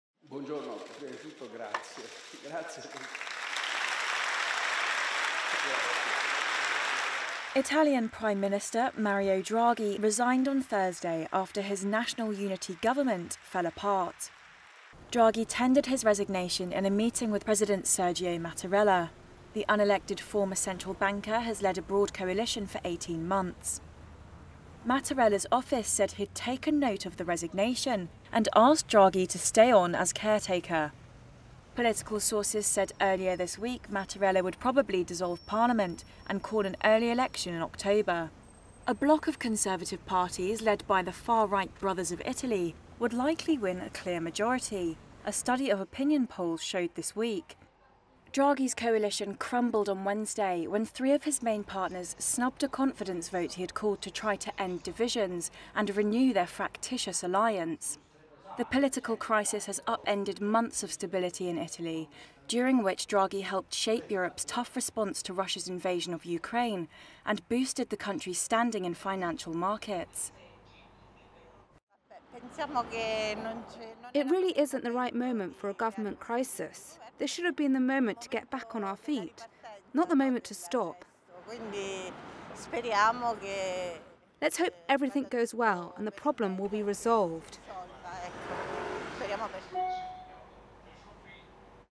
Italian PM resigns after coalition deserts him
Italian Prime Minister Mario Draghi resigned after his national unity government fell apart, setting the country on course for an early election and hitting financial markets.